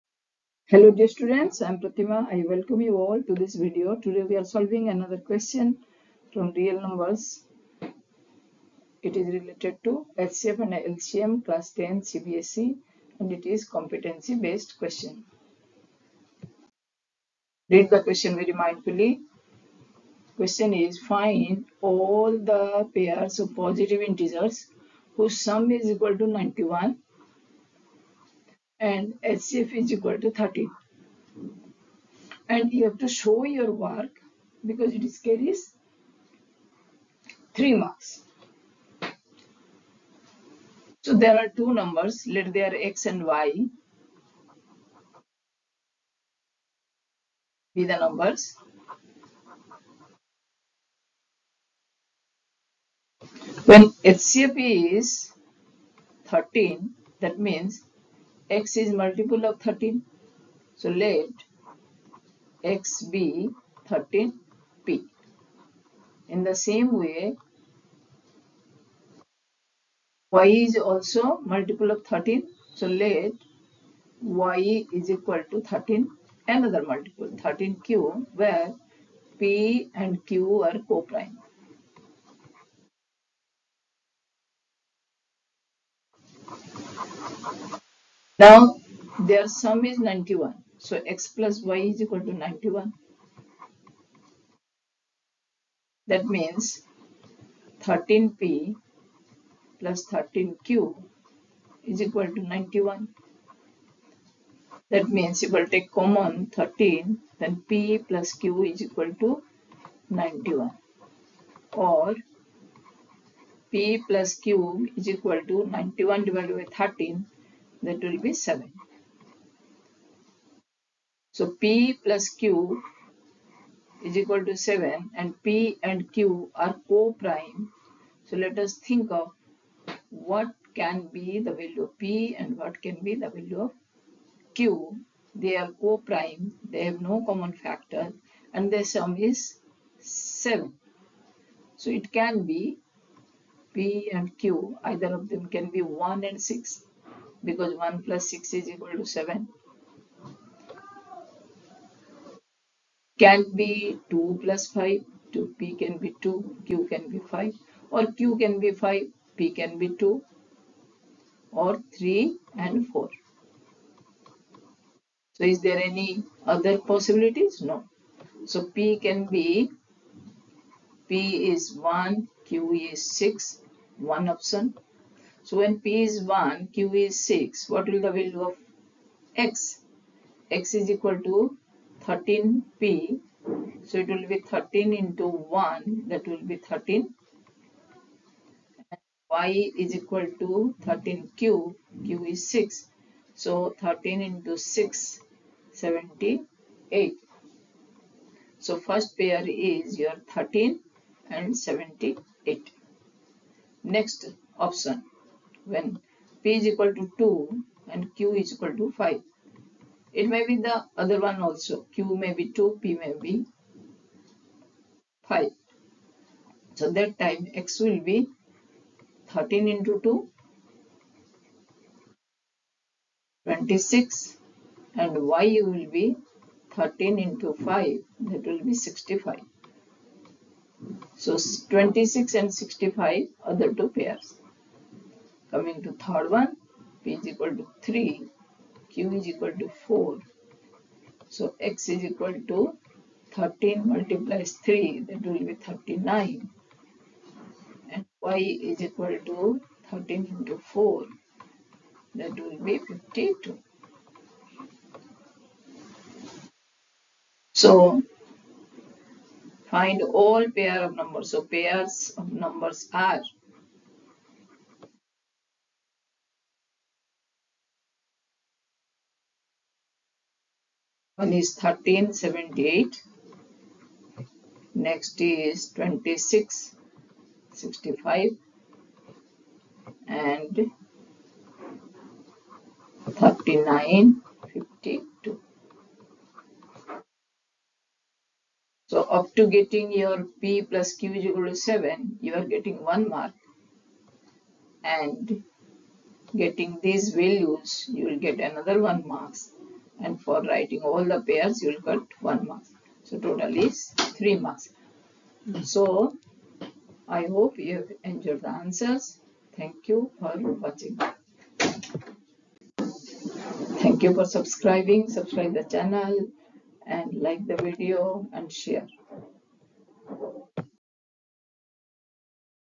A Competency based question | Class 10 | CBSE | Real numbers | Mathematics | HCF | LCM
Find all pairs of positive integers whose sum is 91 and HCF is 13. Show your work.